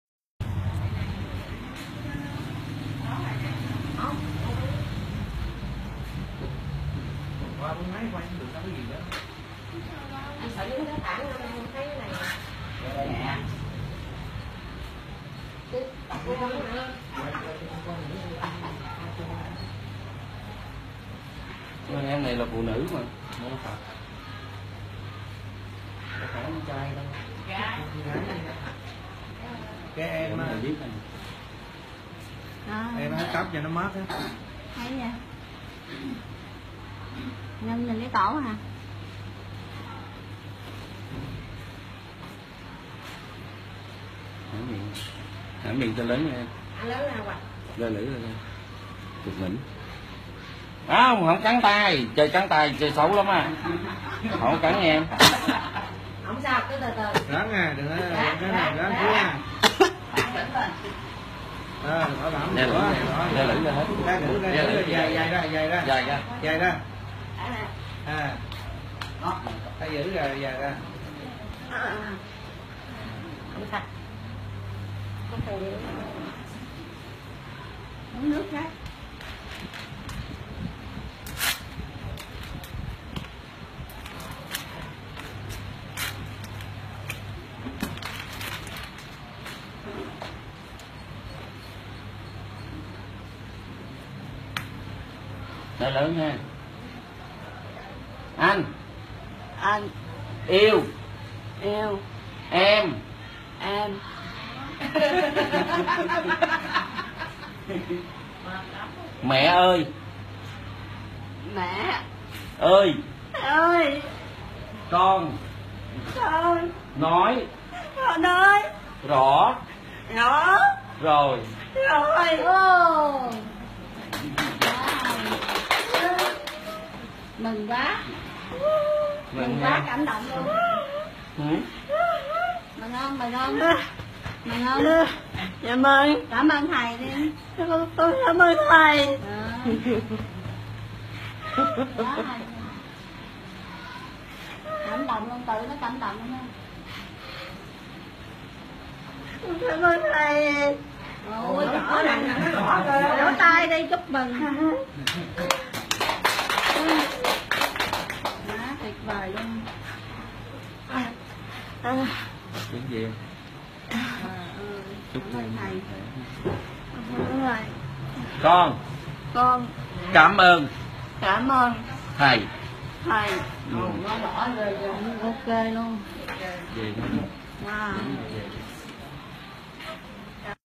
درمان لالی با ماساژ در تایلند
یک تایلندی قادر است با حرکاتی به حالت ماساژ فرد لال را معالجه کند 😳